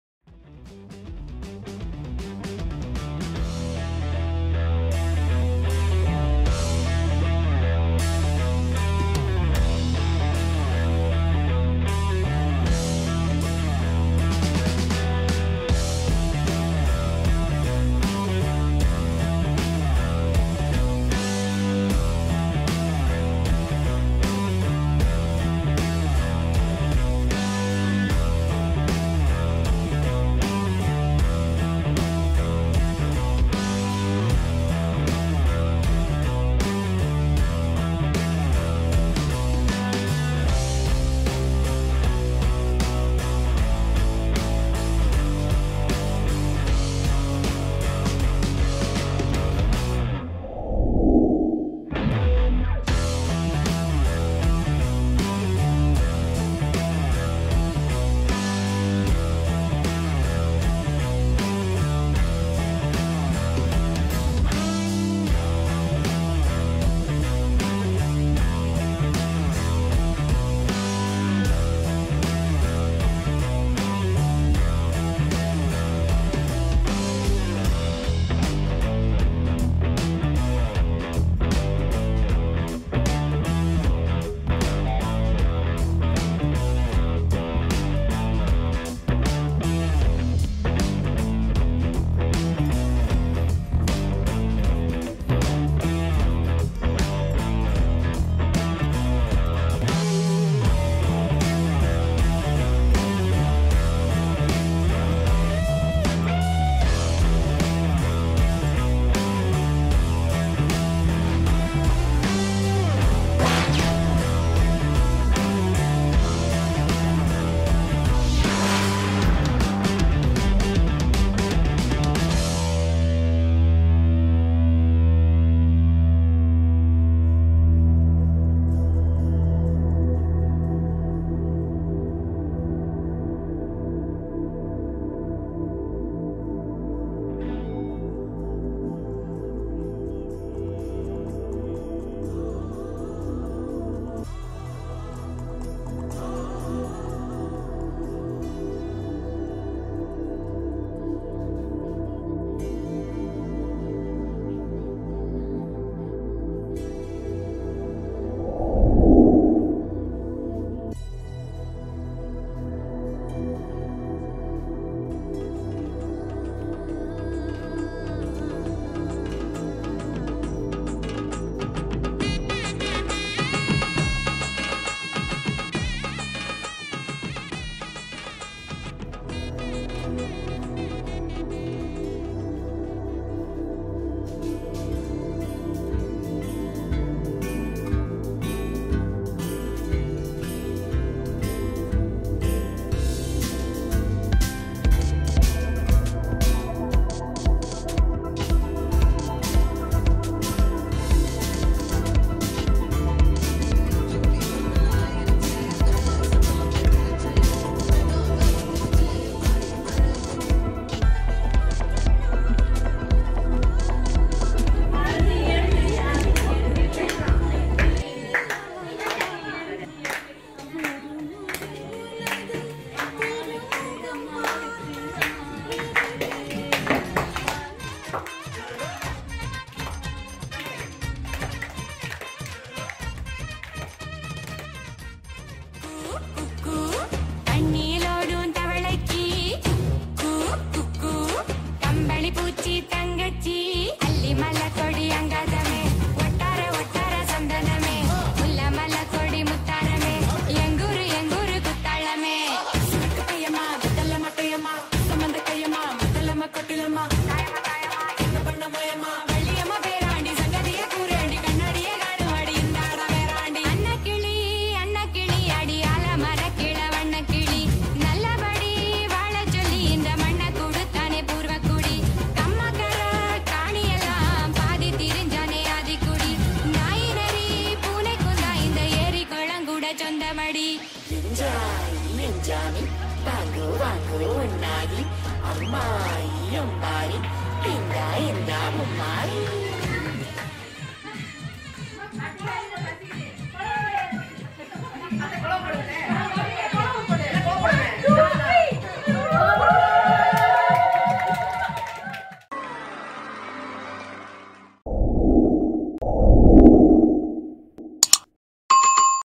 Sweet Memories Of Nalungu | Indian Nalungu Functions | Wedding Nalungu| Dr.Savetha Weds Dr. Arul
Indian wedding are usually considered as sacred ties between two families.Tamil weddings have a set of rituals that are performed during the wedding to make the family members especially ladies happy.
Nalungu the ritual of cleaning and purification is usually done on the night before the actual wedding day.
The kids and ladies come one by one apply turmeric paste on feets,legs,hands,and cheeks of bride.The turmeric paste also brightens the skin colour of the bride.
Some families hold this ritual together with bride and groom.some of them will do separately.
There are games for family members and the ladies sing a song during the arthi functions. Thank you

TimeStamps:
00:00 Intro
00:10 Nattamai welcoming bride
00:31 Special Gifts
00:48 Rose water ritual
02:02 Applying Turmeric Paste
03:05 Cracking the coconut
03:31 Arti function
03:58 Nalungu dance
04:25 Bride celebrations
05:05 Subscribe and Default Settings